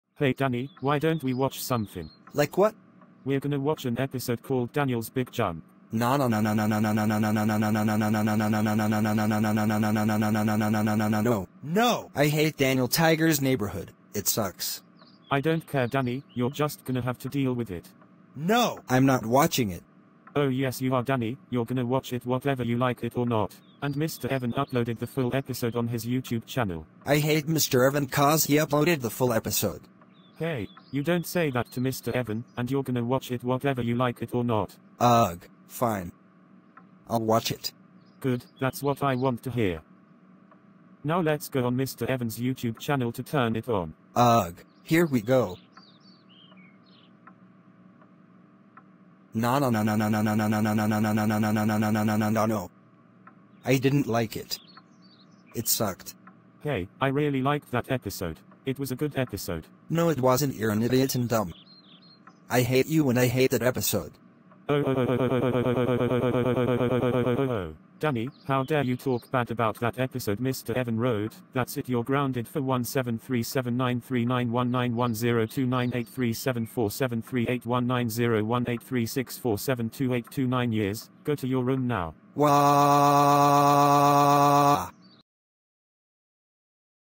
Danny Refuses To Watch Daniel’s Big Jump And Gets Grounded.
This is a video where Danny Refuses To Watch Daniel’s Big Jump And Then He Gets Grounded, Enjoy The Video!